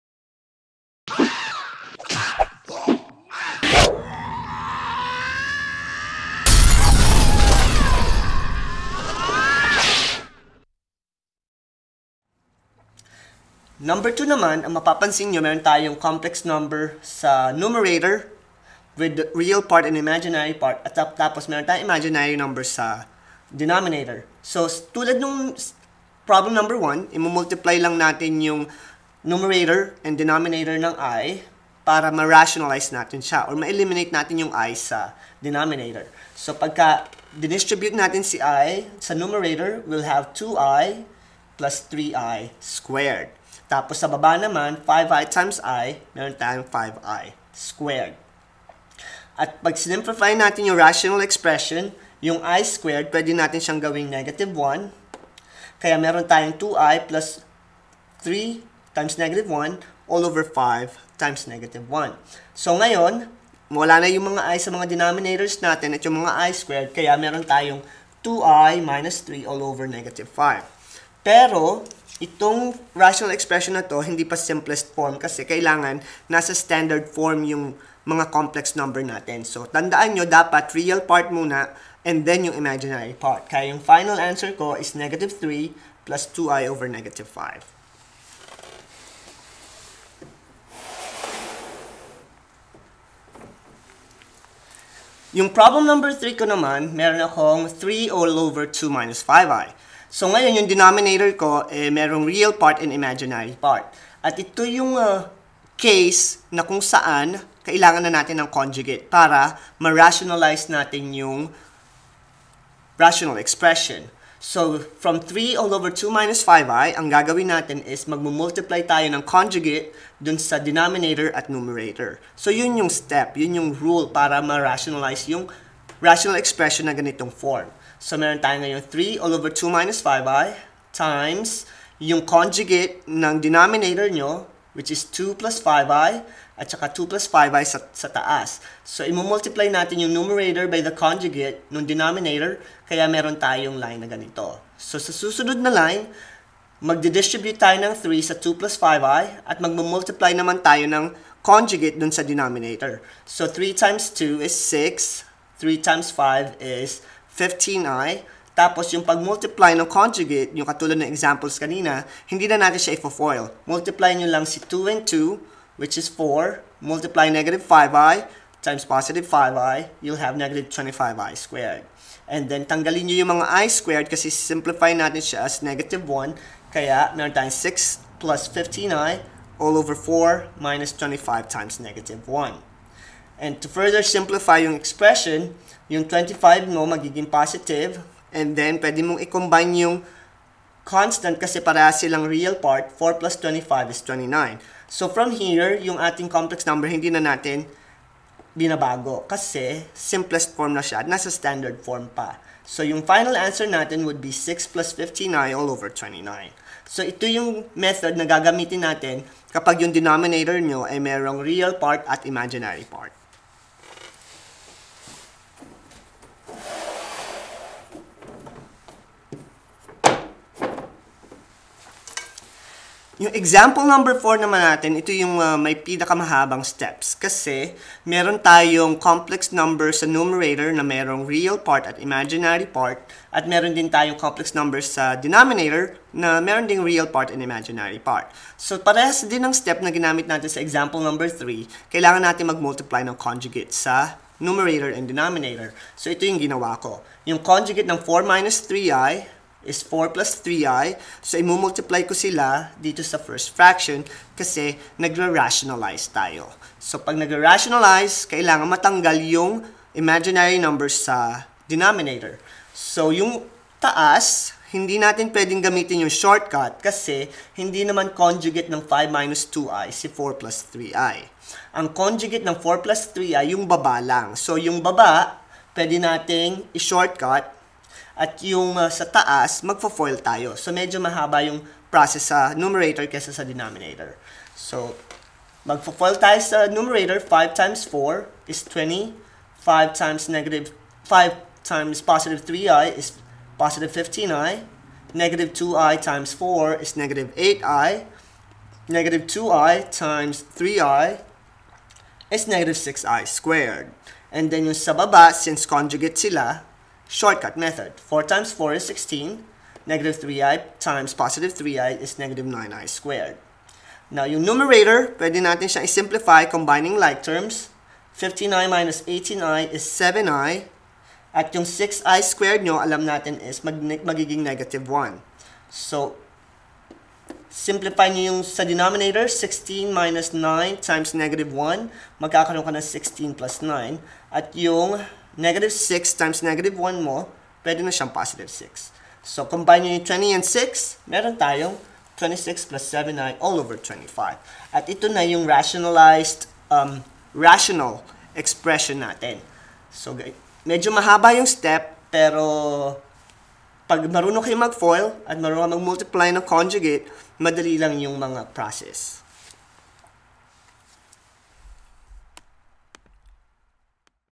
RATIONALIZING COMPLEX NUMBERS | ALGEBRA | PAANO?
Aralin sa algebra tungkol sa pag-rationalize ng mga complex numbers. Ang pagra-rationalize ay isang proseso na ginagamit para mawala ang mga complex numbers sa denominator. Gamit ang mga conjugates ng mga complex numbers, maaring ma-rationalize ang isang expression.

0:14  (2+3i)/5i
1:40 3/(2-5i)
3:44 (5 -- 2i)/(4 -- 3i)